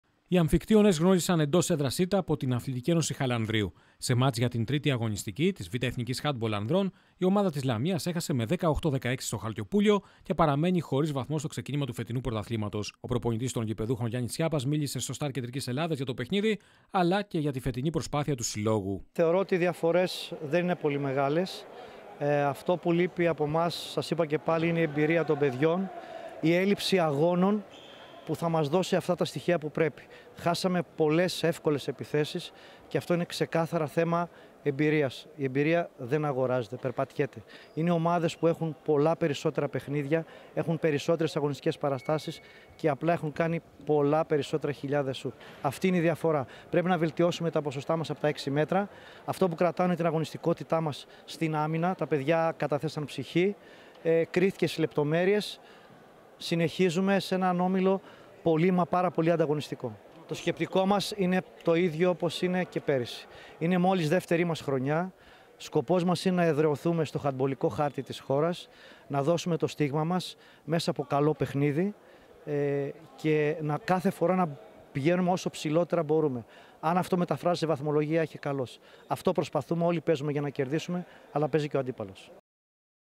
Αμφικτύονες Λαμίας-Α.Ε. Χαλανδρίου 16-18
Για την 3η αγωνιστική της Β΄ Εθνικής χάντμπολ ανδρών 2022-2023.